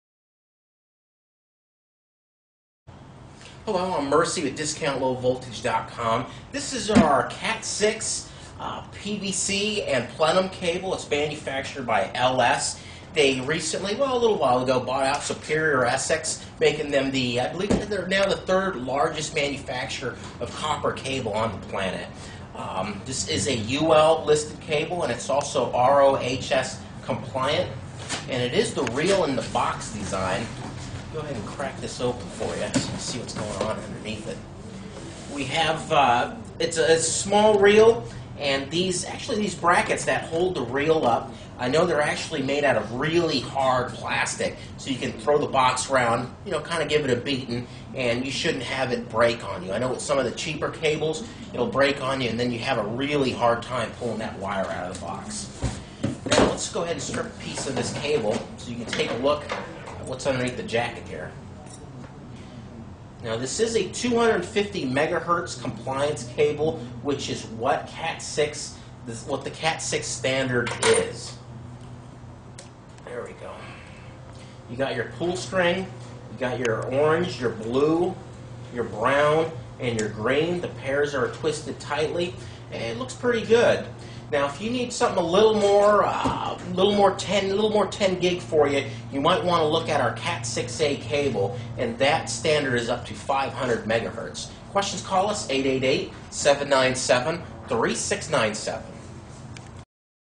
LS Cable Cat6 Bulk Cable Category 6 Ethernet PVC Plenum Cable.flv
Category 6 ethernet cable, aka Cat6, is available in 1000ft bulk cable boxes. LS Cable is a leading manufacturer or low voltage cable, and is our preferred brand of cable. We have been selling Cat5e and Cat6 LS Cable for over 4 years. You can see the complete line of LS Cable products